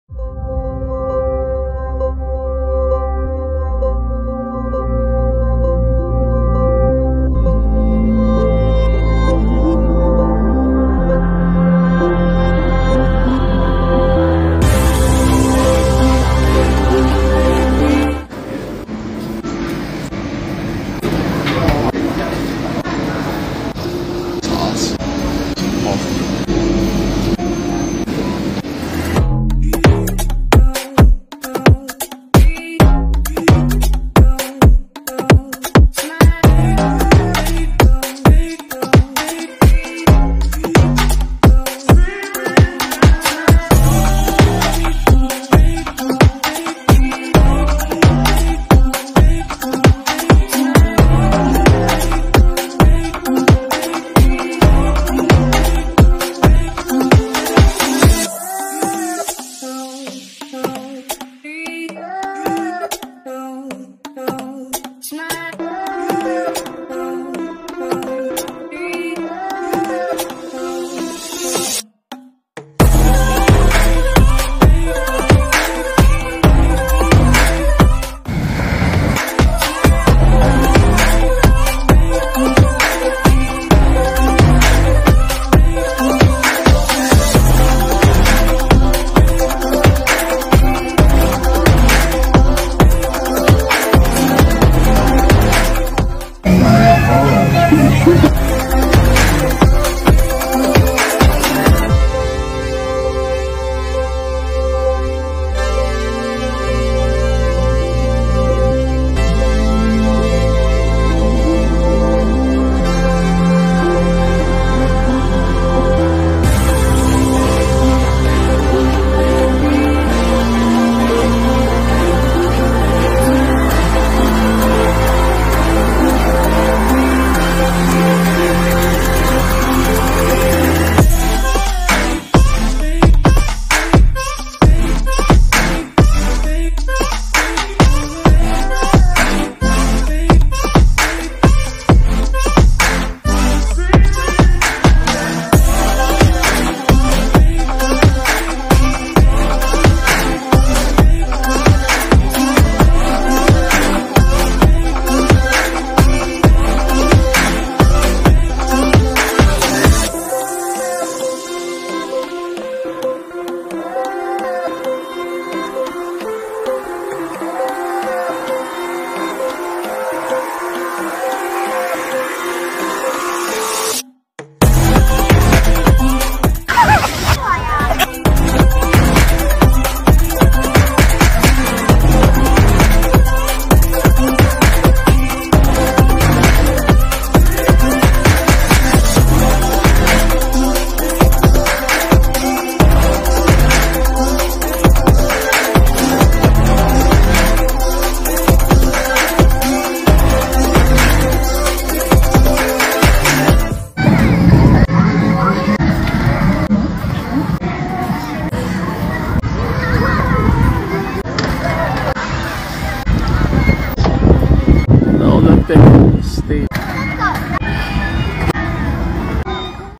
Shenyang Olympic Sports Center
Shenyang Olympic Sports Center's International Ice and Snow Carnival offers a variety of rides and games suitable for tourists of different ages. Visitors will enjoy themselves here from dawn to dusk. People experiencing snowy mountain surfing in the center will slide hand in hand together, and feel a rush of adrenaline when rushing down the slope. The ice slide is 12 meters high and 20 meters long. Sitting in a basket and going down the ice slide, visitors feel like they are back to the winter camp they expected most as children.